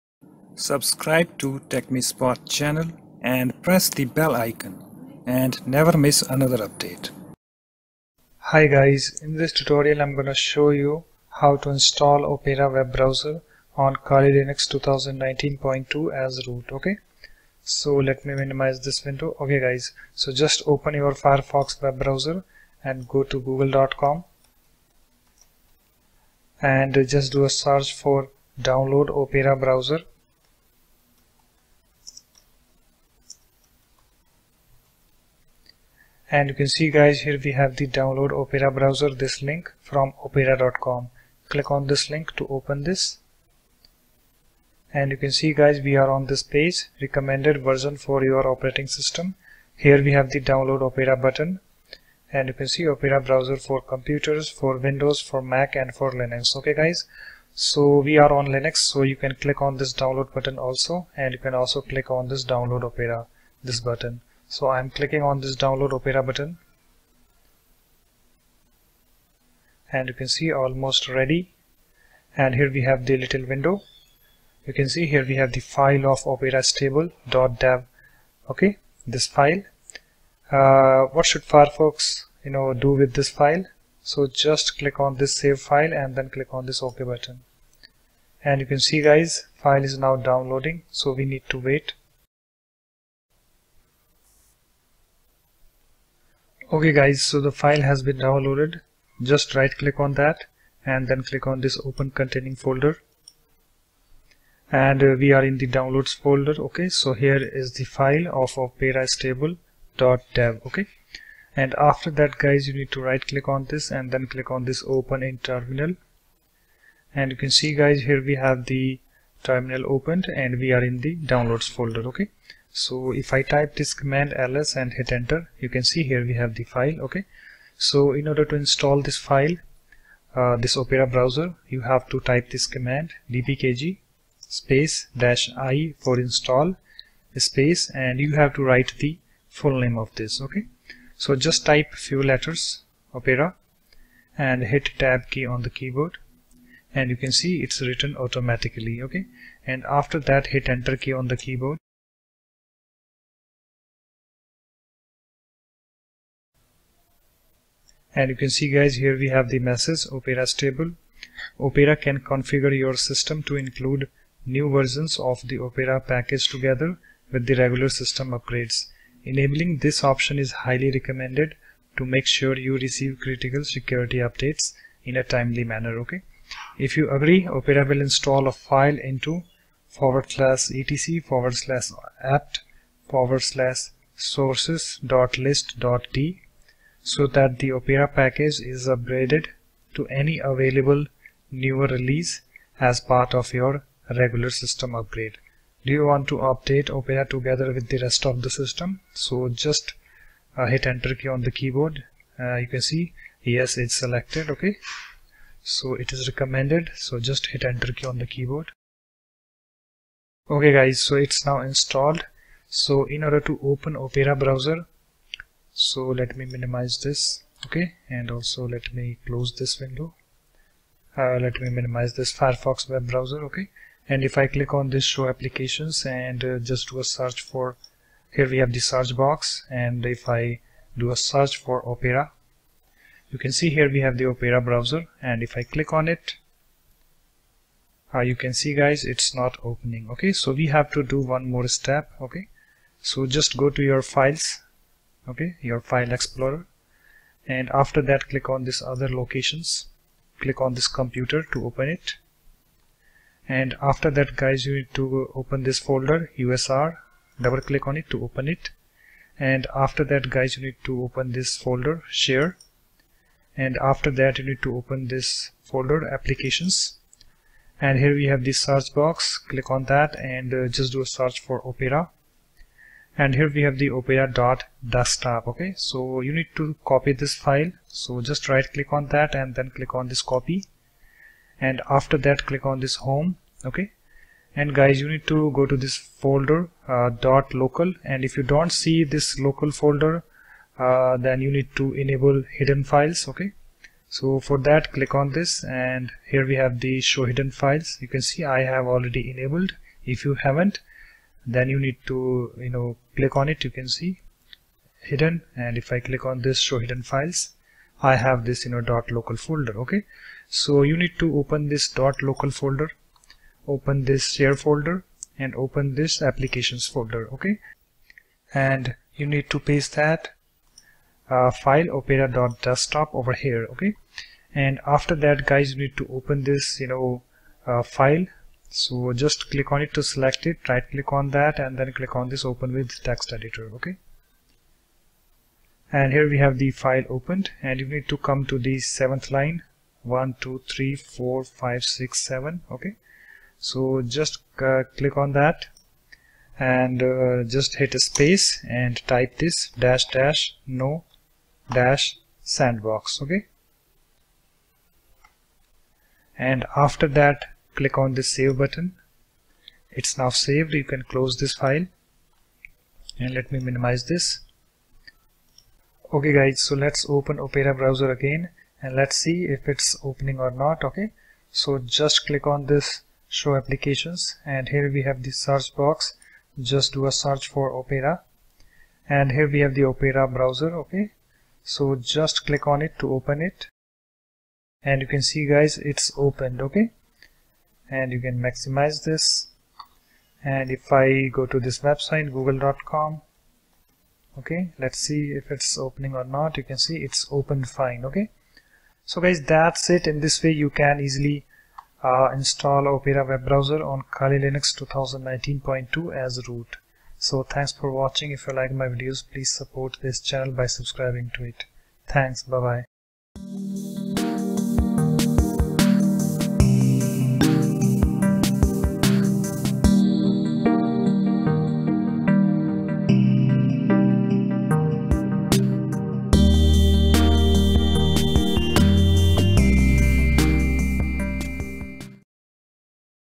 How to Install Opera Browser on kali Linux 2019.2 as Root?
Step 2: Do a search for 'Download Opera Browser', and then open the first link.

Step 3: Click on the 'Download Opera' button.

Step 4: On the Pop Up, click on 'save file', and then click on 'ok' button.

Step 5: Go the folder where you have downloaded the .deb file.

Step 6: Right click on it, and then click on 'open in terminal'.

Step 7: Now, Type the command below to install it:-

        dpkg -i full_file_name

and then hit enter key on the keyboard.

Step 8: Select 'Yes' on the message, and then hit enter.

Step 9: After installing, close the terminal.

Step 10: Now, Click on 'Files' in the dock, and then click on 'other locations'.

Step 11: Click on 'Computer'

Step 12: Now, go to 'usr/share/applications', and then do a search for 'opera.desktop', copy the file.

Step 13: Now, Click on 'home', and then go to '.local/share/applications' paste the 'opera.desktop' file there.

Step 14: Open the 'opera.desktop' file, and then go to the seventh line.

Step 15: write '--no-sandbox'  without quotes next to Exec=opera %U with space, and then save the file.

Step 16: Go to the 'show applications' in dock, and do a search for 'opera', click on it to open it.

It's done